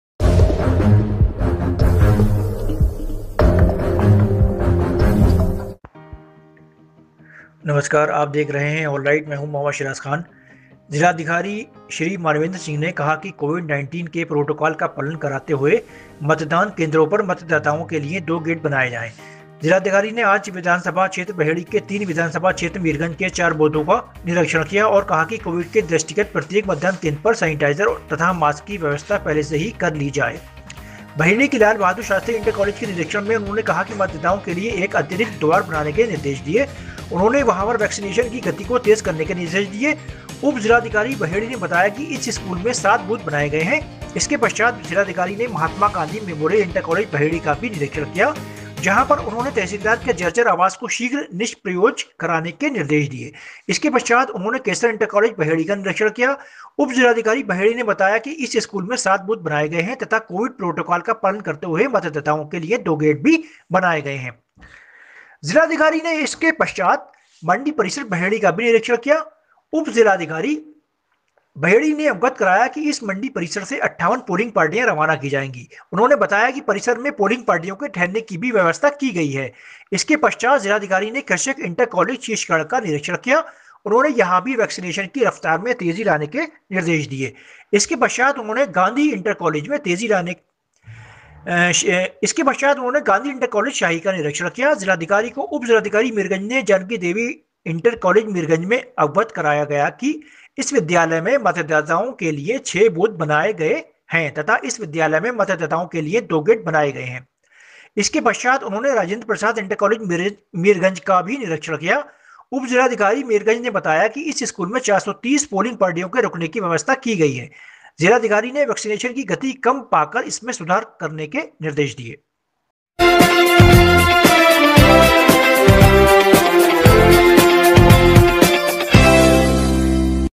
Bareilly News- मतदान केन्द्रों पर मतदाताओं के लिये दो गेट बनाए जाएं : ज़िलाधिकारी
बरेली, 11 जनवरी। ज़िलाधिकारी/ज़िला निर्वाचन अधिकारी श्री मानवेंद्र सिंह ने कहा कि कोविड-19 के प्रोटोकॉल का पालन कराते हुए मतदान केन्द्रों पर मतदाताओं के लिये दो गेट बनाए जाएं। ज़िलाधिकारी ने आज विधानसभा क्षेत्र बहेड़ी के 3, विधानसभा क्षेत्र मीरगंज के 4 बूथों का निरीक्षण किया और कहा कि कोविड के दृष्टिगत प्रत्येक मतदान केन्द्र पर सैनेटाइज़र तथा मास्क की व्यवस्था पहले से ही कर ली जाए। बहेड़ी के लाल बहादुर शास्त्री इण्टर कालेज के निरीक्षण में उन्होंने कहा कि मतदाताओं के लिये एक अतिरिक्त द्वार बनाने के निर्देश दिए। उन्होंने वहां पर वैक्सीनेशन की गति को तेज़ करने के निर्देश दिए। उप ज़िलाधिकारी बहेड़ी ने बताया कि इस स्कूल में 7 बूथ बनाये गये हैं। इसके पश्चात ज़िलाधिकारी ने महात्मा गांधी मेमोरियल इण्टर कालेज बहेड़ी का भी निरीक्षण किया जहां पर उन्होंने तहसीलदार के जर्जर आवास को शीघ्र निष्प्रयोज्य कराने के निर्देश दिये। इसके पश्चात उन्होंने केसर इण्टर कालेज बहेड़ी का निरीक्षण किया। उप ज़िलाधिकारी बहेड़ी ने बताया कि इस स्कूल में 7 बूथ बनाये गये हैं तथा कोविड प्रोटोकाल का पालन करते हुये मतदाताओं के लिये 2 गेट भी बनाये गये हैं। ज़िलाधिकारी ने इसके पश्चात मण्डी परिसर बहेड़ी का भी निरीक्षण किया। उप ज़िलाधिकारी बहेड़ी ने अवगत कराया गया कि इस मण्डी परिसर से 58 पोलिंग पार्टियां रवाना की जायेंगी। उन्होंने बताया कि परिसर में पोलिग पार्टियों के ठहरने की भी व्यवस्था की गई है। इसके पश्चात ज़िलाधिकारी ने कृषक इण्टर कालेज शीशगढ़ का निरीक्षण किया। उन्होंने यहां भी वैक्सीनेशन की रफ्तार में तेज़ी लाने के निर्देश दिए। इसके पश्चात उन्होंने गांधी इण्टर कालेज शाही का निरीक्षण किया। ज़िलाधिकारी को उप ज़िलाधिकारी मीरगंज ने जानकी देवी इंटर कालेज मीरगंज में अवगत कराया गया कि इस विद्यालय में मतदाताओं के लिये 6 बूथ बनाए गए हैं तथा इस विद्यालय में मतदाताओं के लिये 2 गेट बनाये गये हैं। इसके पश्चात उन्होंने राजेन्द्र प्रसाद इण्टर कालेज मीरगंज का निरीक्षण किया। उप ज़िलाधिकारी मीरगंज ने बताया गया कि इस स्कूल में 430 पोलिंग पार्टियों के रुकने की व्यवस्था की गयी है। ज़िलाधिकारी ने वैक्सीनेशन की गति कम पाकर इसमें सुधार करने के निर्देश दिये।